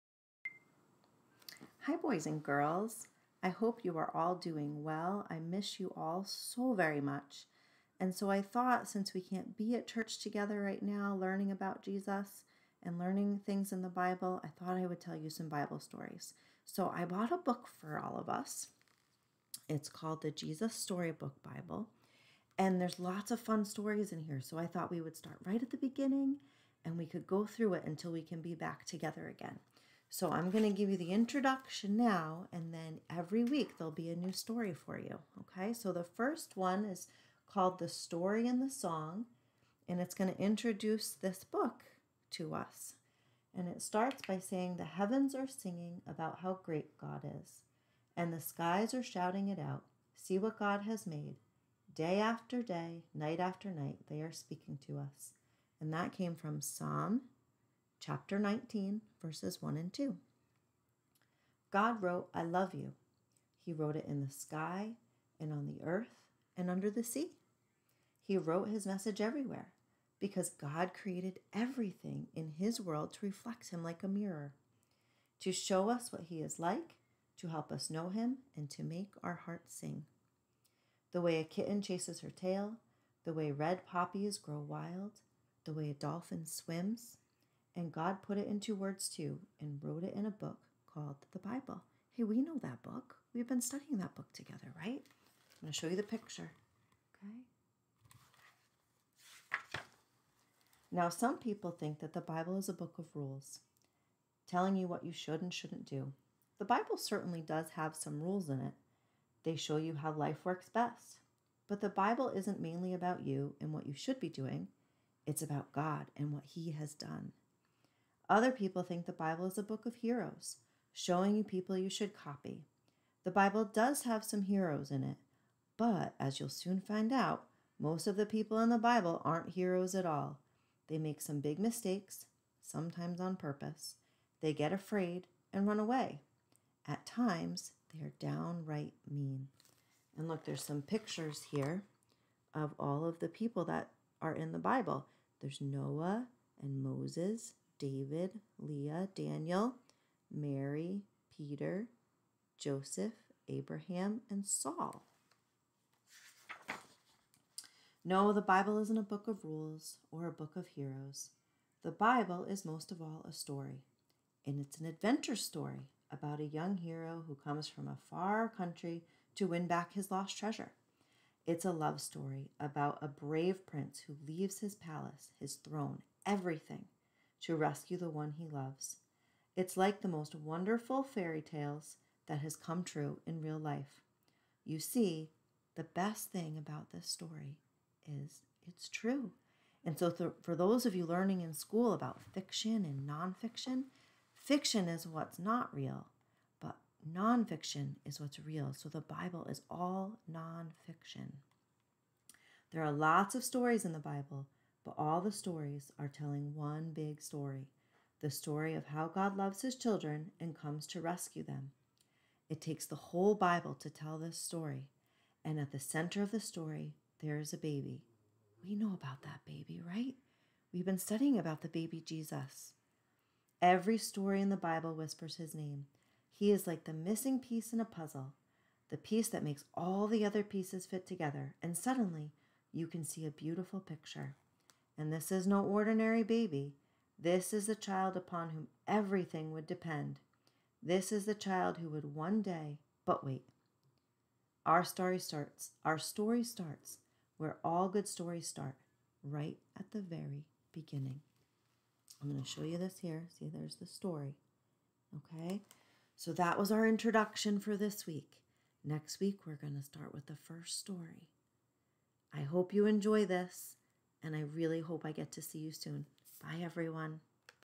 Jesus Storybook Bible Introduction 6-7-2020
Welcome to Kid's Corner! Ms. Amy reads a story from the Jesus Storybook Bible
 6-7-2020